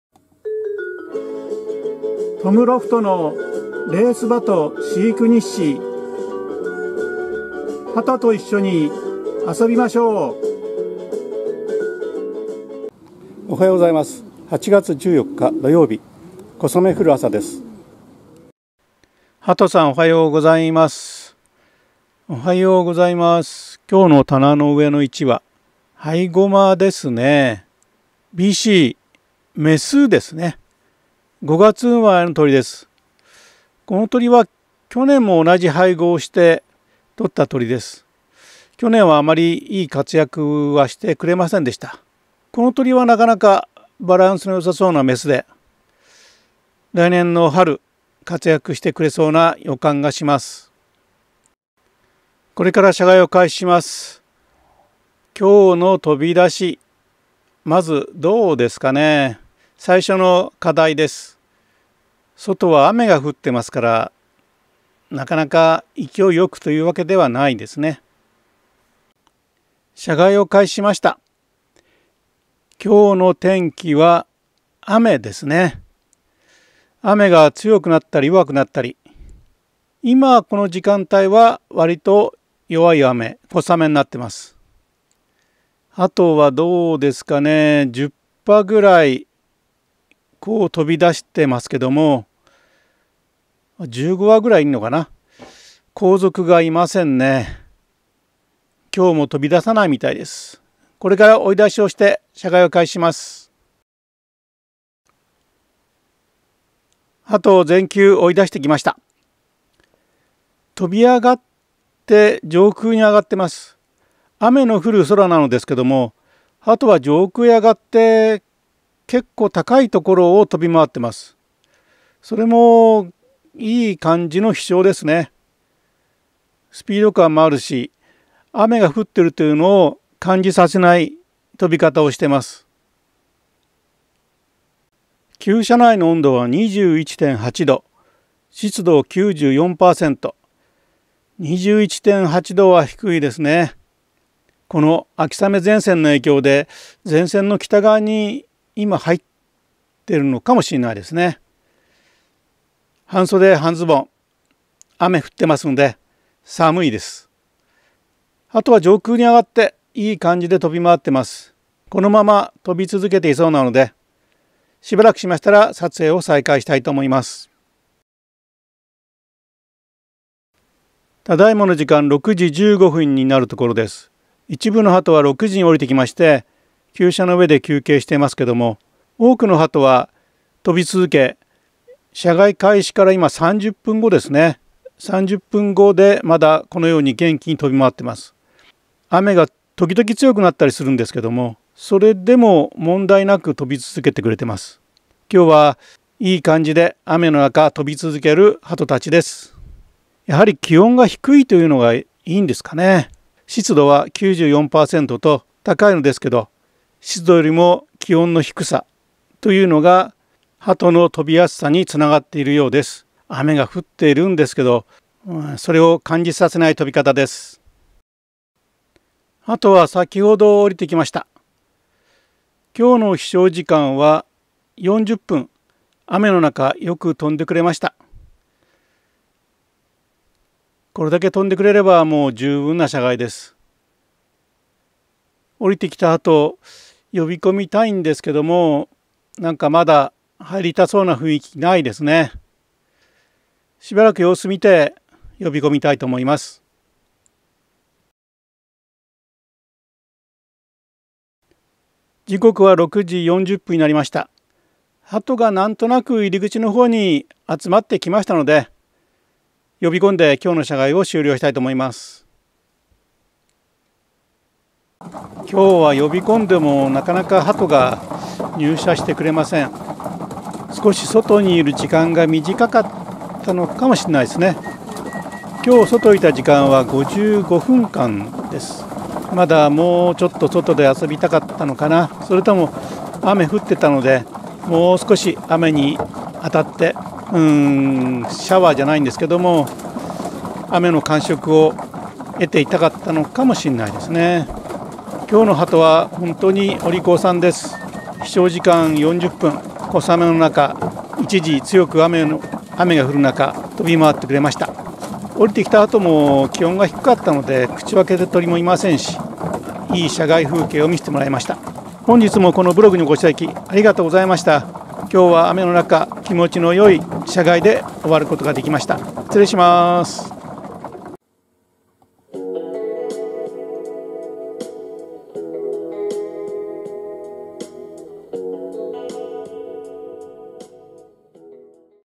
21 8 14（土）雨の舎外
雨の中の舎外は何回目？
雨降りの空よく飛んでいました。